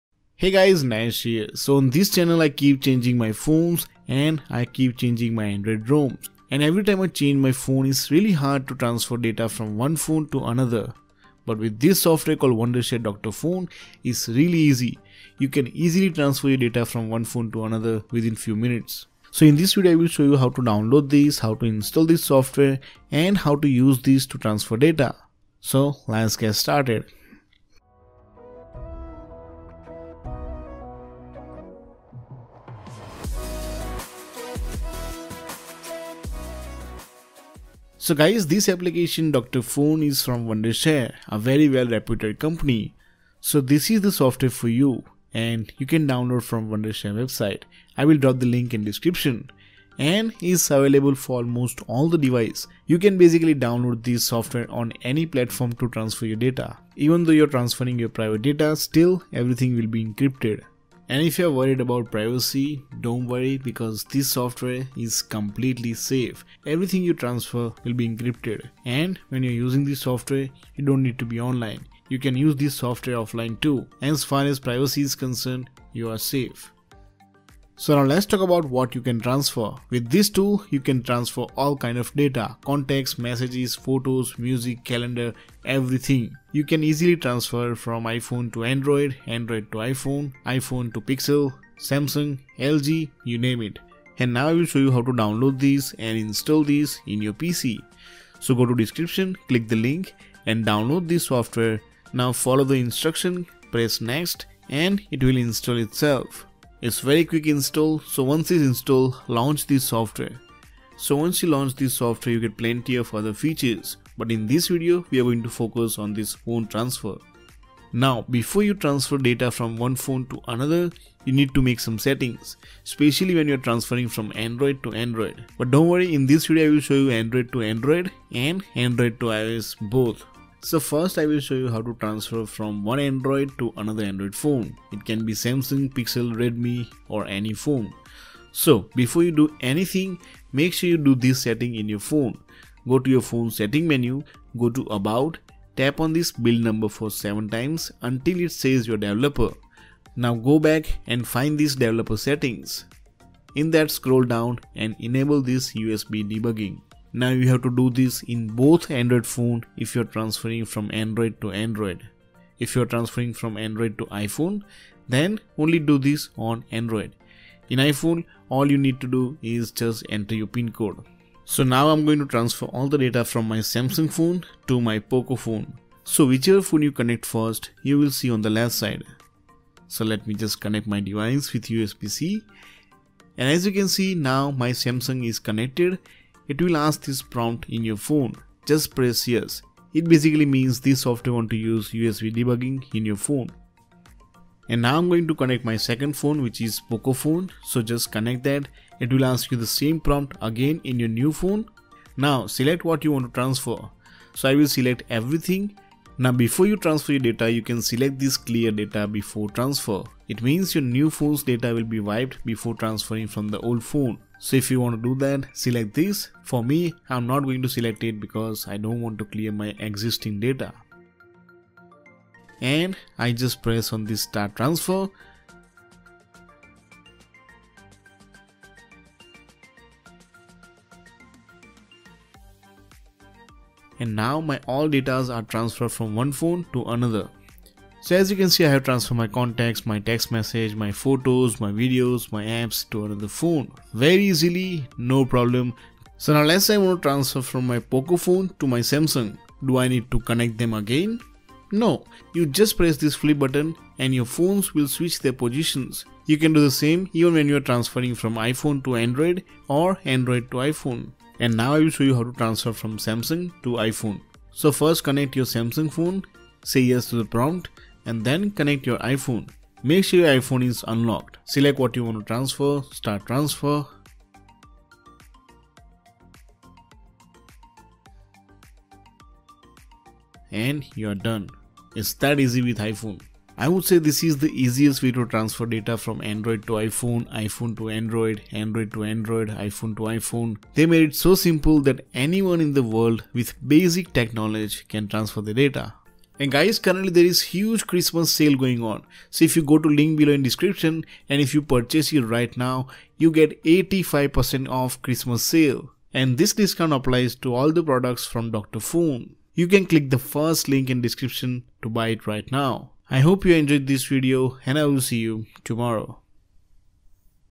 How to Transfer Apps + Data Between Phones Dr Fone Wondershare
Dr.Fone Phone Transfer tool enables you to transfer all types of data from one phone to another in one-click .